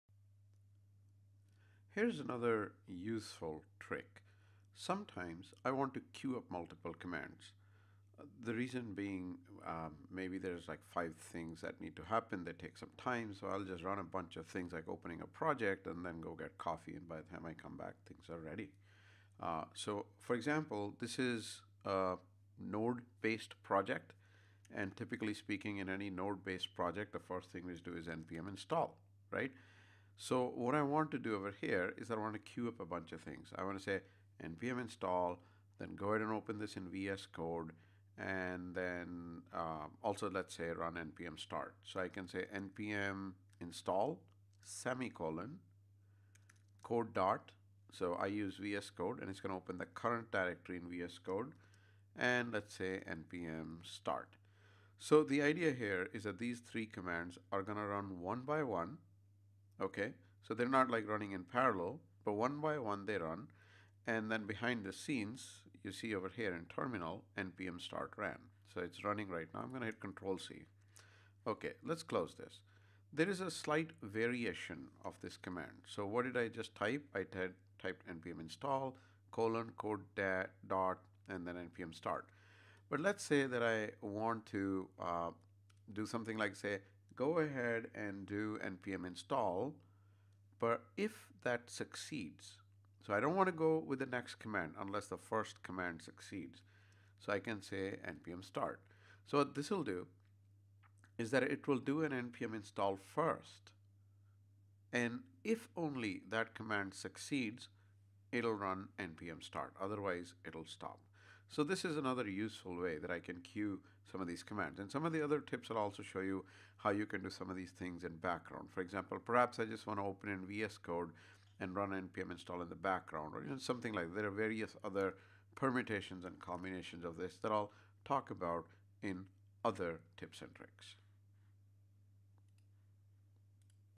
Terminal trick: Running multiple commands
A mac/linux terminal trick that lets you run multiple commands